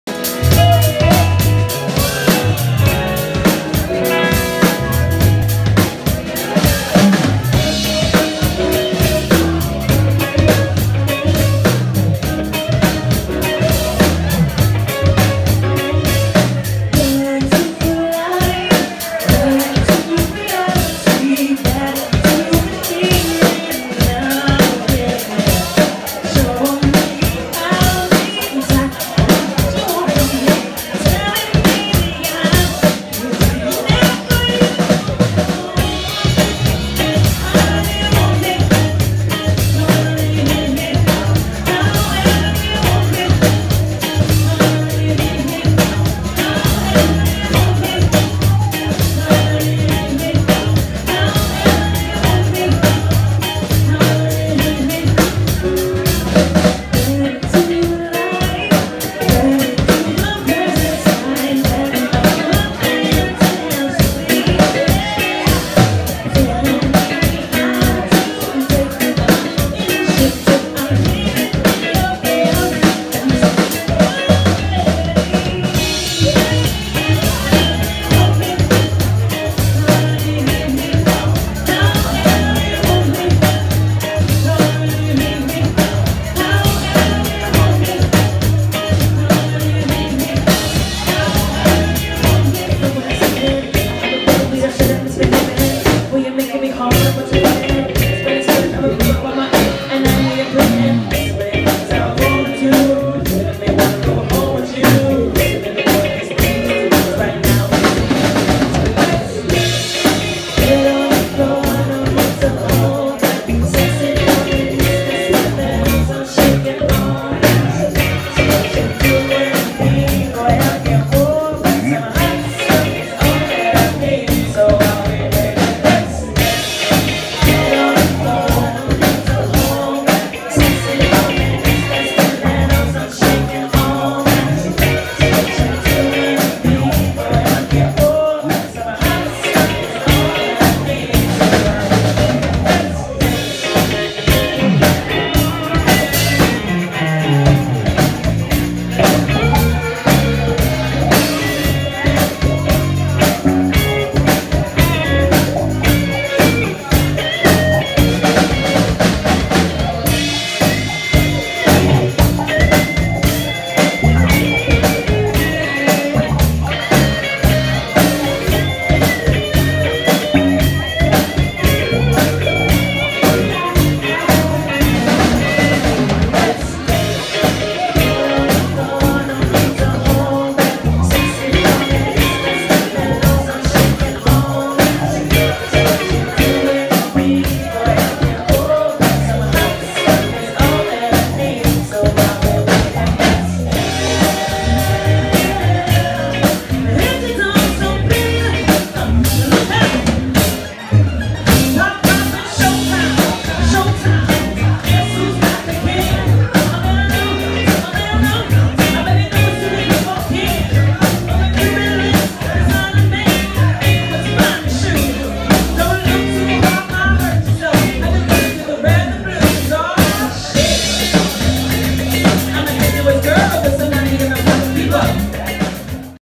GYLES AT ALLEYCATZ FRIDAY NOVEMBER 29, 2019
UPTOWN @ ALLEY CATZ  ONE OF THE BEST LIVE SPOT IN TORONTO

LivePerformance By: GYLES

AlleyCatz 2409 YONGE ST TORONTO ON

LOOK OUT FOR ALLEYCATZ NEW YEARS EVE PARTY